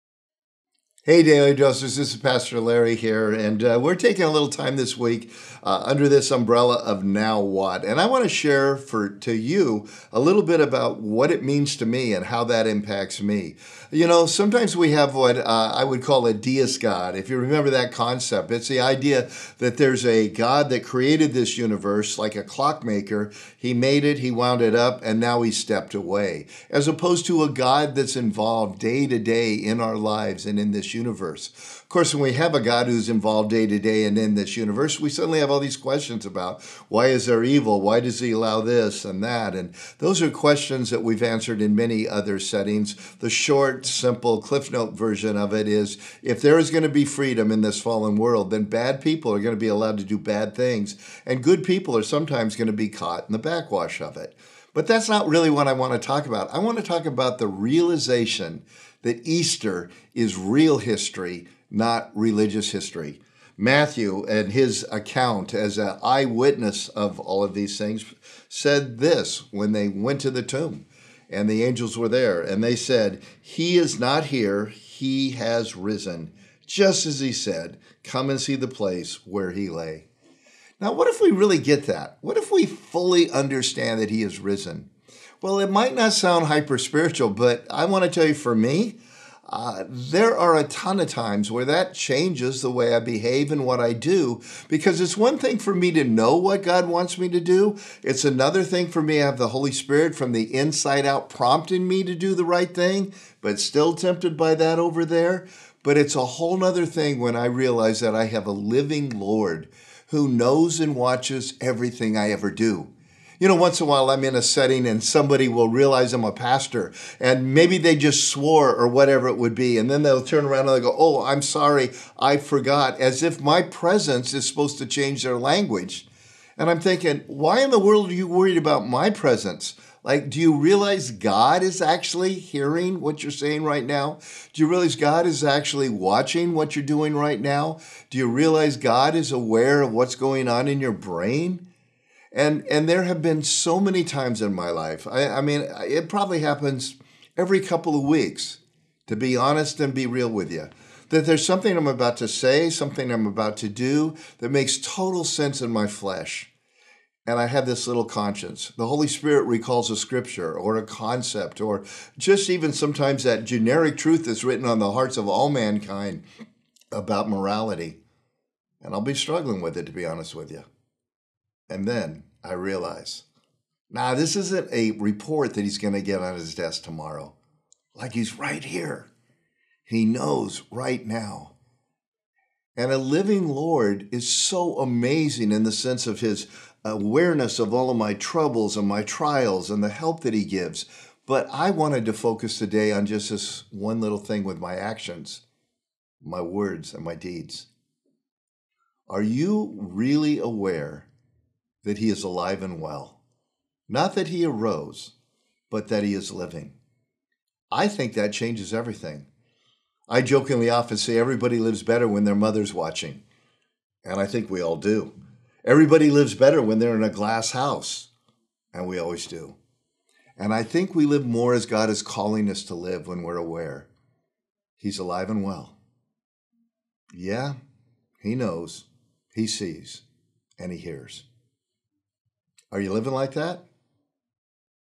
Now What? Part 5 - Daily Dose
Pastor Larry Osborne continues our daily devotional series Now What?
Friday, April 14th Daily Dose. The topic of today's dose changes everything.

0:00 Intro
1:03 Matthew 28:1-6
1:53 Morality
3:11 Aware